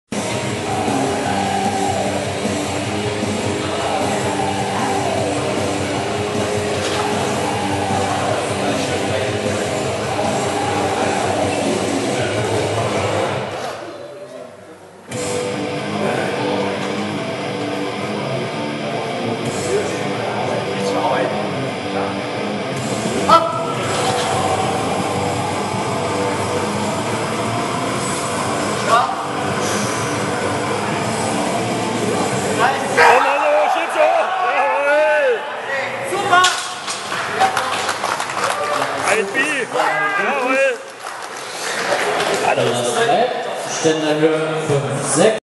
Hanno Tettenborn 200 kg
Hanno Tettenborn ( KBV Bautzen) drückt 200 kg in der 1. Runde der Sachsenoberliga im Bankdrücken in Eibau am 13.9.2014